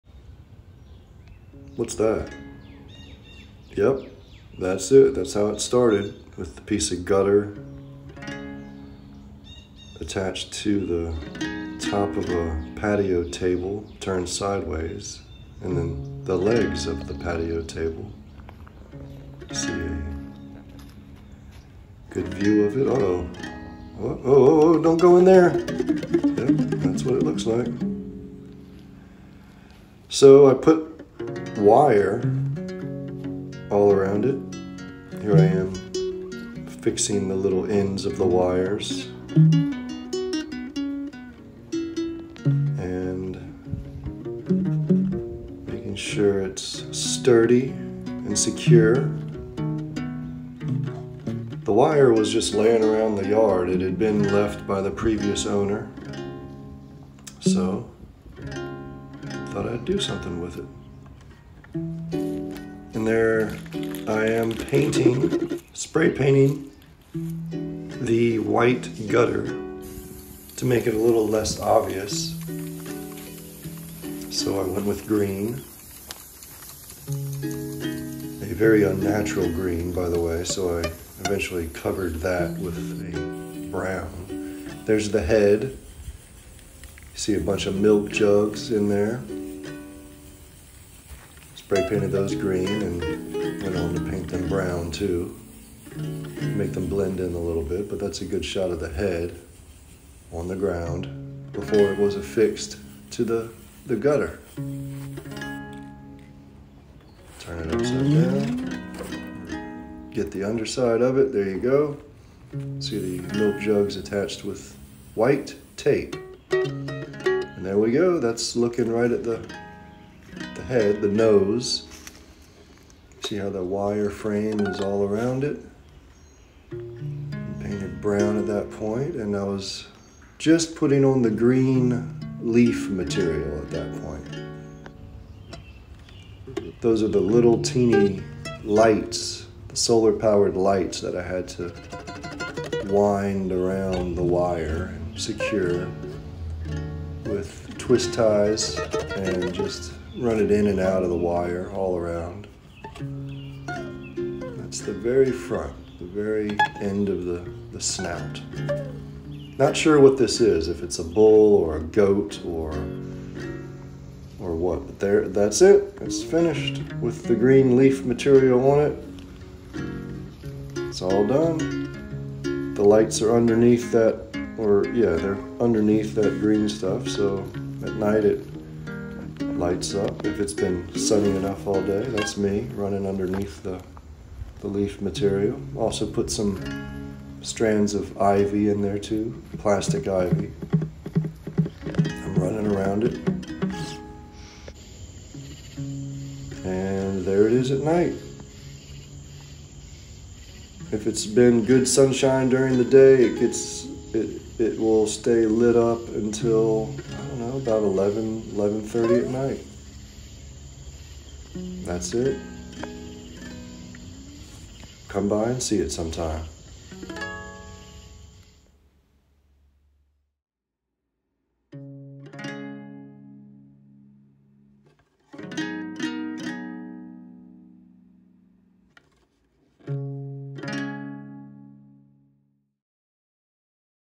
I Made A Backyard Wire Thing
Made with wire, tape, garden hose, gutter piece, patio table pieces, spray paint, twist ties, little lights, green leaf material, and plastic ivy.

In September 2024, I opened the back of its head and extracted all the plastic jugs. They made the head too heavy and it, along with the neck, would tilt over if hit by a strong gust of wind. I also strengthened the neck to the wire frame with some zip ties.

In December 2024, strong winds were still making the neck tilt and would occasionally cause the entire sculpture to fall on its side. I added a long bamboo pole to the neck and concealed it under the green leaf material. It’s zip tied firmly in place. I also had to weigh the entire thing down with a heavy umbrella stand. It’s attached underneath with a bungee cord. I’ll have to replace that with another heavy object when we want to use the umbrella this Spring.